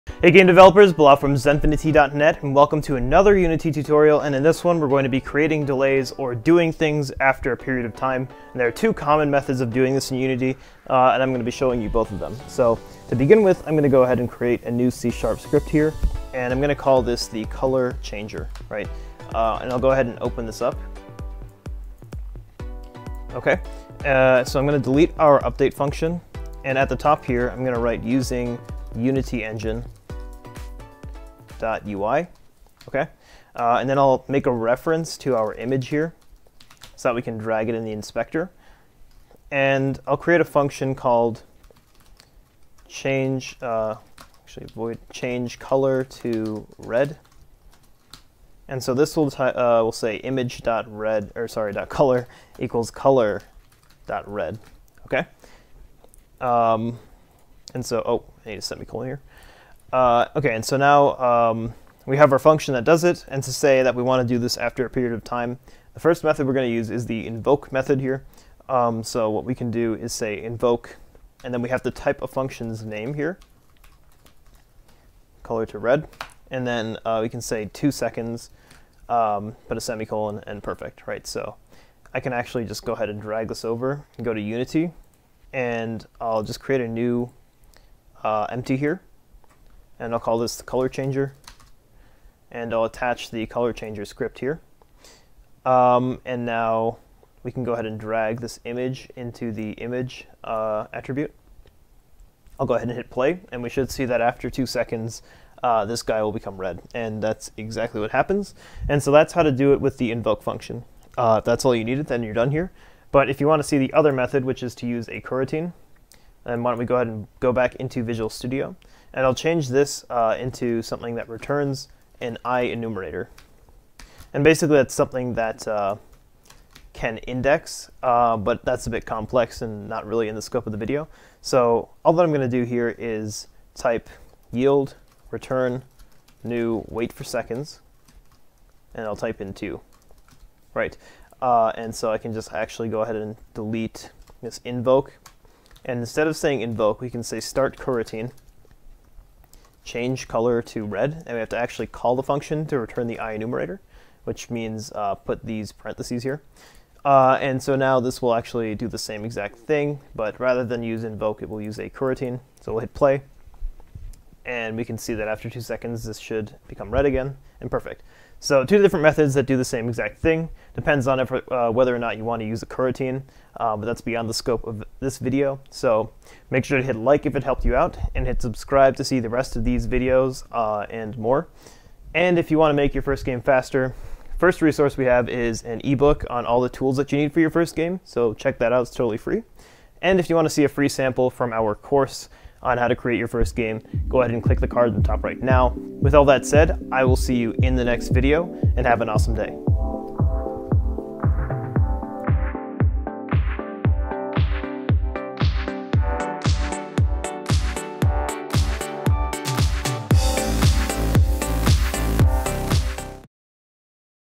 Do After Time - Creating Delays in Unity | Unity Do Something After Time Tutorial
FREE Make Your First Game E-Book!

P.S. If you want to make your first game, DON'T wait!